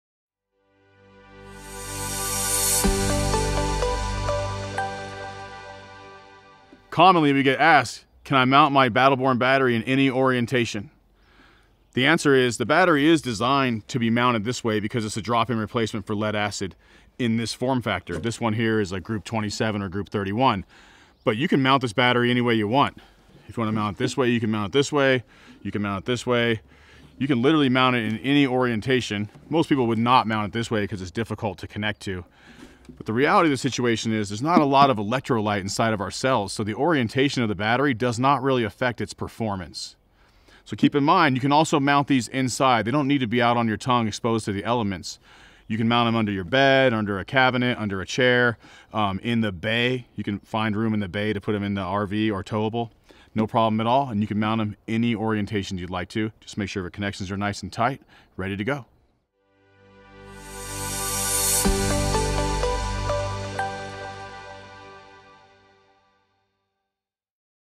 FAQ: Can LiFePO4 batteries be mounted in any position? | Battle Born Batteries
At Battle Born Batteries, we get a lot of questions about how to position our products! In this new FAQ video, our COO Sean demonstrates how the batteries can be mounted in your system in any way that works best for your system. You can read more about our batteries at the latest blog post in our FAQ series